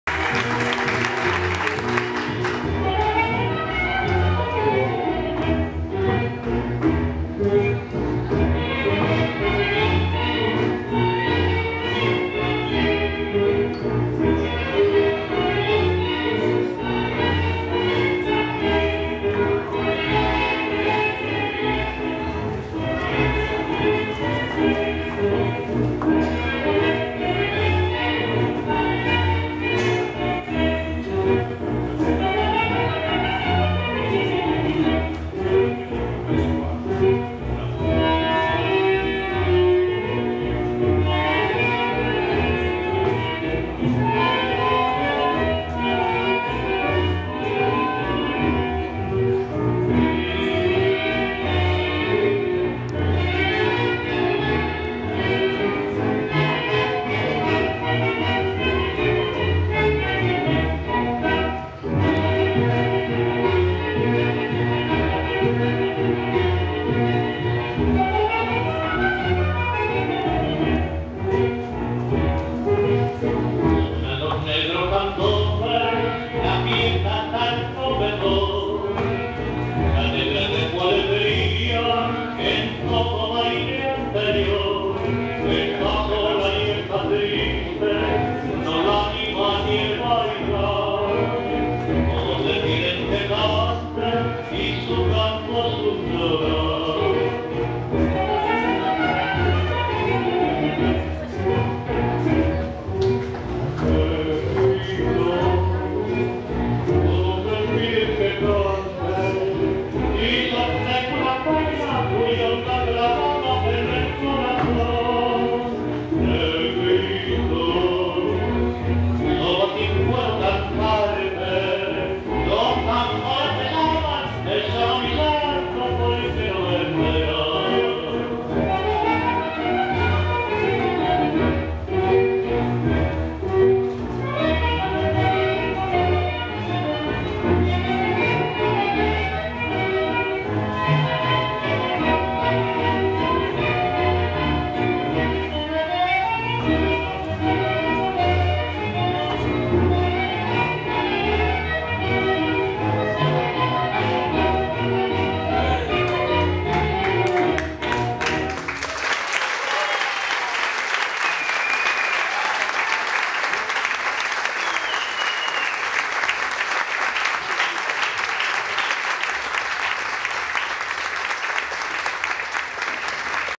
GASTON GODOY E LAURA ATIENZA NA MILONGA SUPERESPECIAL
Nas Galerias de Paris, Porto, em 11/06/16.